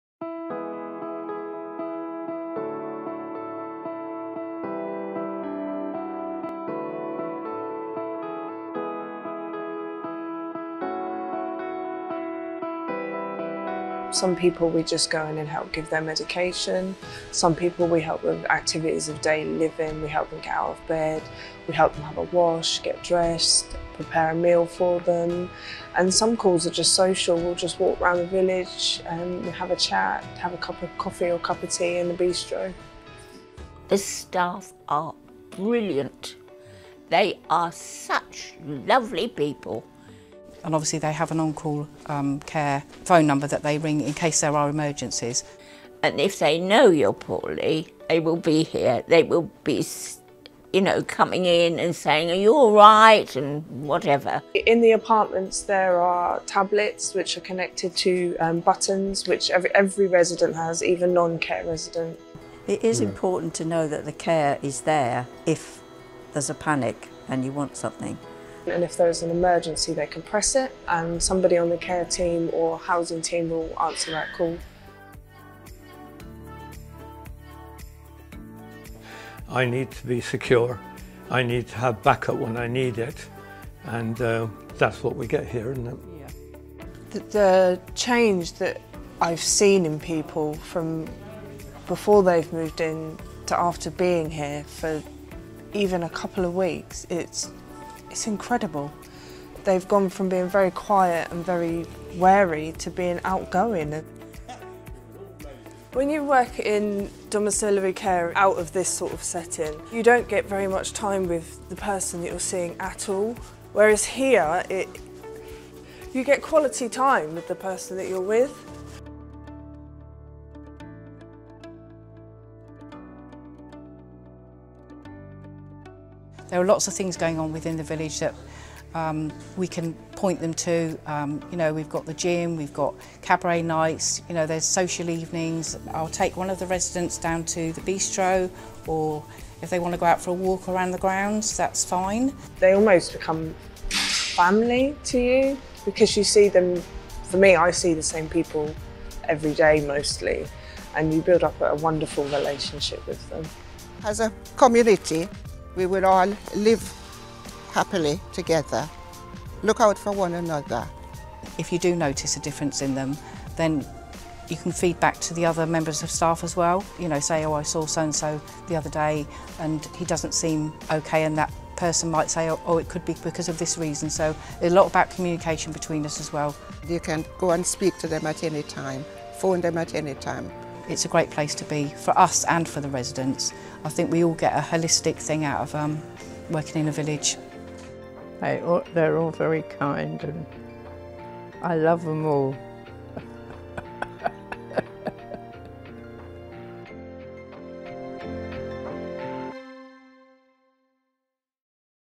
Care is at the Heart of Everything
Within Integrated Retirement Communities, residents have access to high-quality, personalised care whenever they need it. They can increase or decrease the amount of care they receive based on their individual circumstances. 88% of residents say the provision of onsite care was a ‘positive’ or ‘very positive’ factor behind their move.

In this video we hear more about why the care provided in Integrated Retirement Communities is so special, and why it’s at the heart of everything